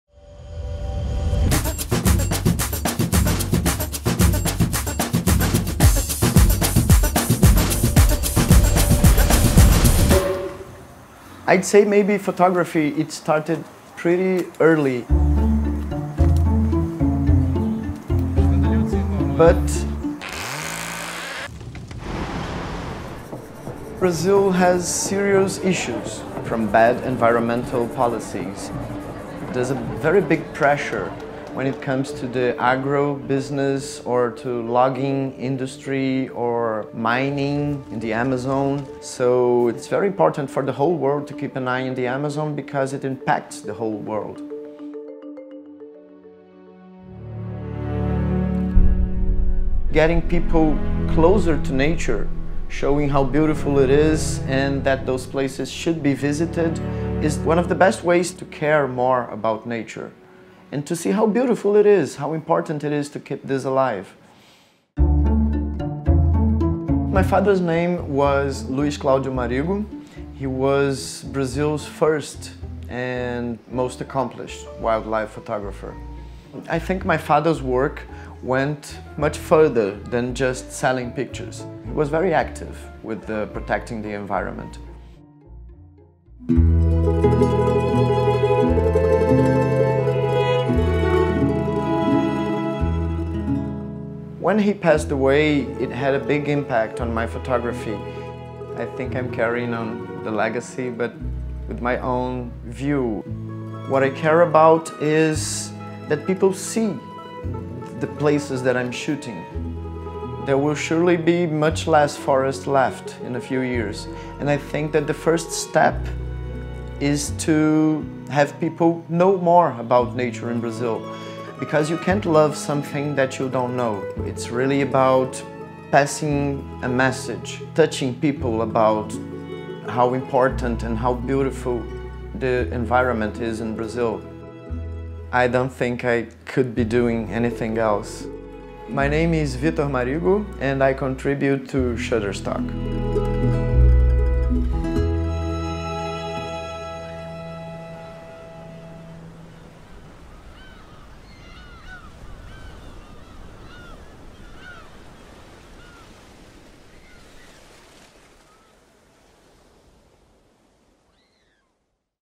Shutterstock Presents: Artist Series with Vitor Marigo
| Nature conservation in Brazil has never been more important than it is today. And in Rio de Janiero, we meet a conservation photographer whose work focuses on protecting this land. That photographer- is Vitor Marigo.

Growing up, Vitor Marigo’s relationship with his father shaped his creativity into what it is today. Like father, like son—he followed his father's footsteps and became a successful photographer in his own right, focusing on conservationist and environmentalist causes in his native Brazil. When his father passed, a spark was lit in Vitor to explore the raw beauty of  Brazil. Today, he preserves nature through photography in an era when it’s more critical than ever. This is Vitor Marigo.

P.S This film was made using Handpainted Stroke techniques.